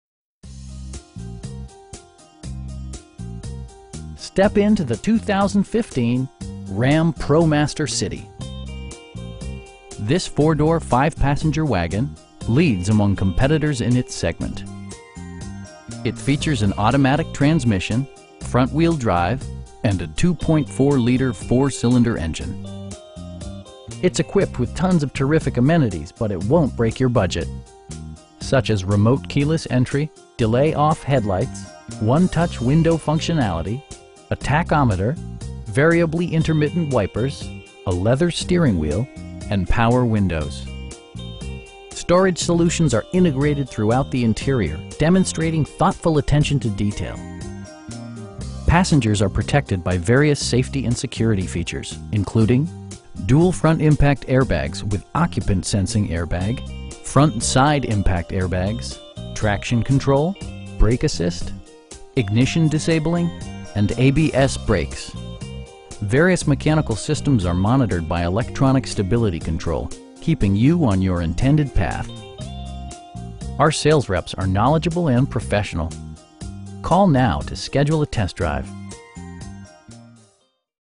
2015 Ram ProMaster City Tradesman in Victoria, TX 77901
Killebrew Inc.

Step into the 2015 Ram ProMaster City. This 4 door, 5 passenger wagon leads among competitors in its segment. It features an automatic transmission, front-wheel drive, and a 2.4 liter 4 cylinder engine. It's equipped with tons of terrific amenities, but it won't break your budget. Such as remote keyless entry, delay-off headlights, 1-touch window functionality, a tachometer, variably intermittent wipers, a leather steering wheel, an overhead console, and power windows. Storage solutions are integrated throughout the interior, demonstrating thoughtful attention to detail. Passengers are protected by various safety and security features, including: dual front impact airbags with occupant sensing airbag, front SIDE impact airbags, traction control, brake assist, ignition disabling, and ABS brakes. Various mechanical systems are monitored by electronic stability control, keeping you on your intended path. Our sales reps are knowledgeable and professional. We'd be happy to answer any questions that you may have.